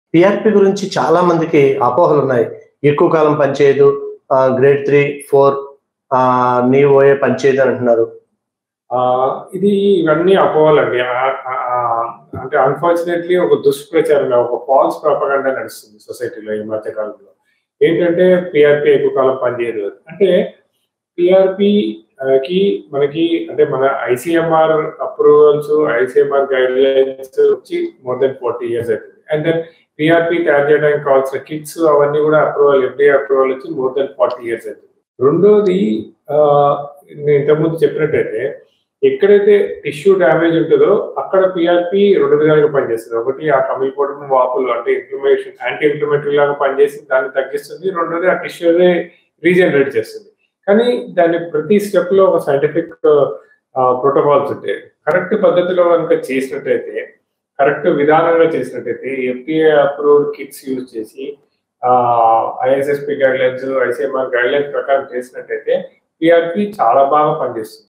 పి ఆర్ పి మీద వున్న అనుమానాలు అపోహలు..! : Dr.Sudhakar | Epione Pain Management | Hyderabad || VSB TV
పి ఆర్ పి మీద వున్న అనుమానాలు అపోహలు మాత్రమే..! : Dr.Sudhakar | Epione Pain Management | Hyderabad || VSB TV